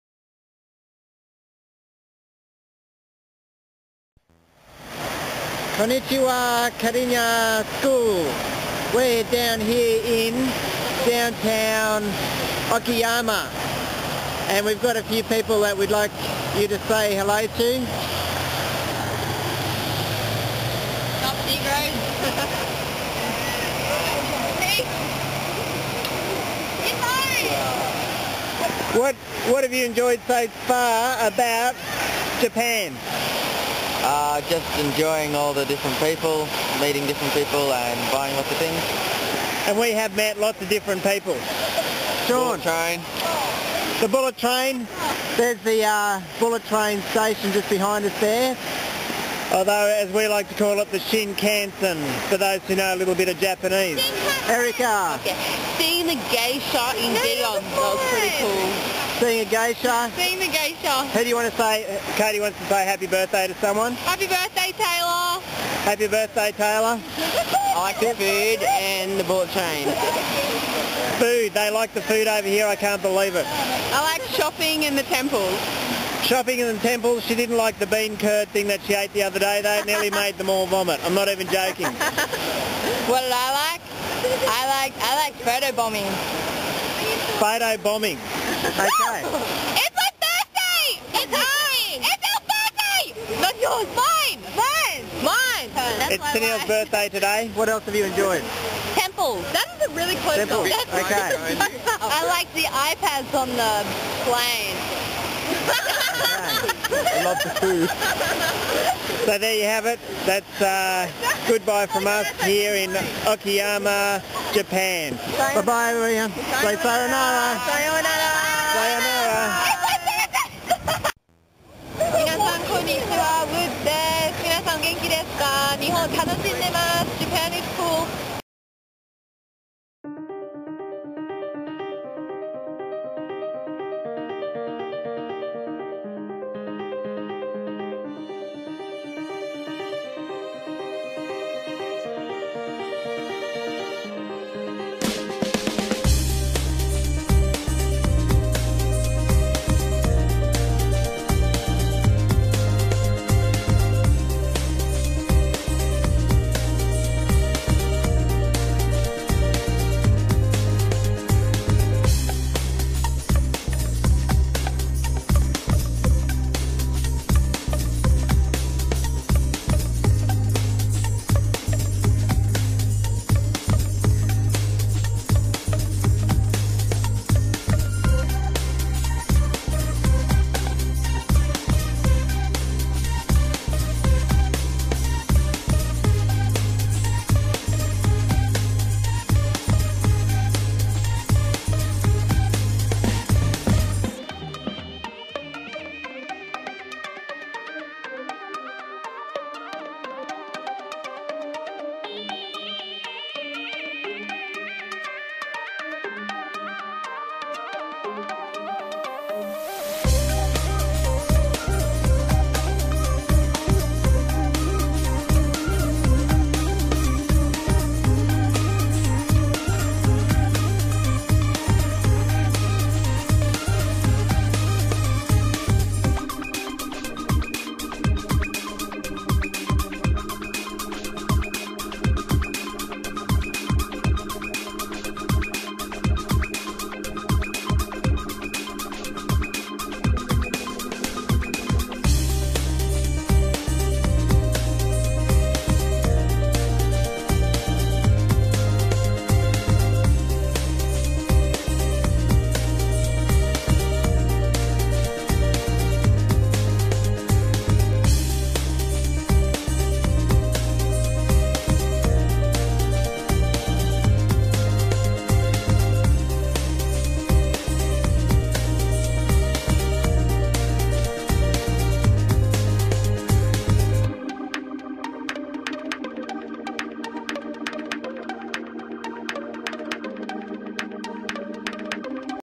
Japan Trip 2012
A video from students and teachers visiting Japan in 2012